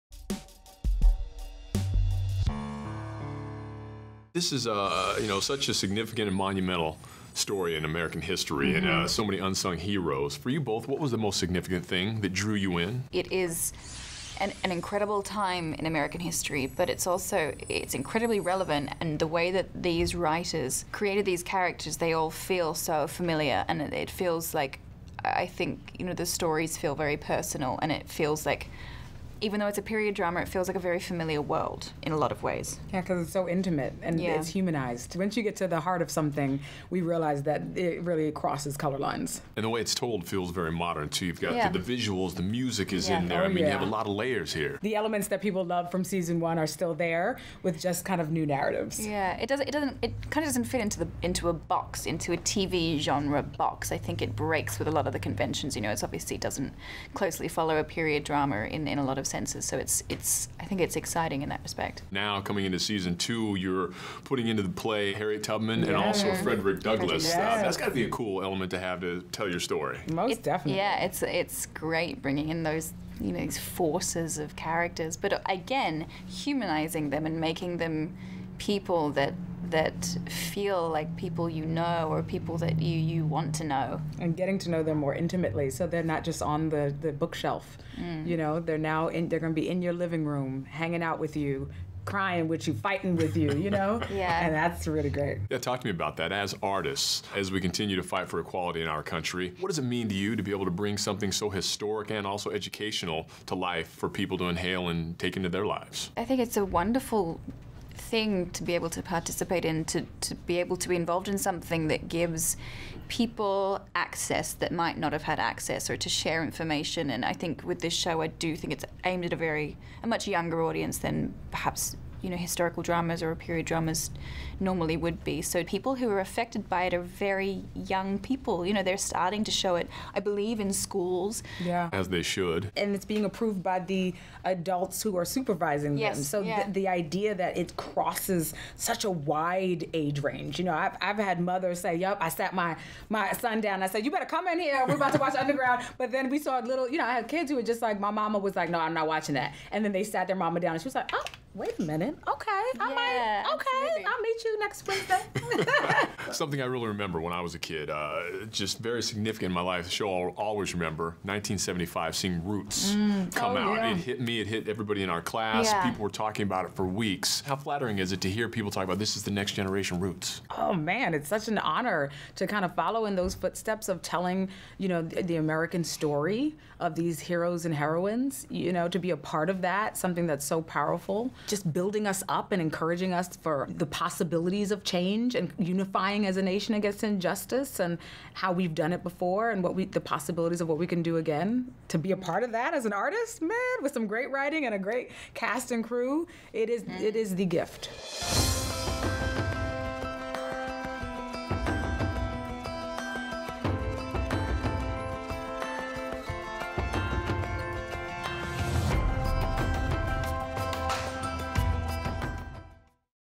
ES Archive "Underground" Jessica De Gouw & Amirah Vann interview
Jon Kelley interviews Jessica De Gouw & Amirah Vann for "Underground" from January 27, 2017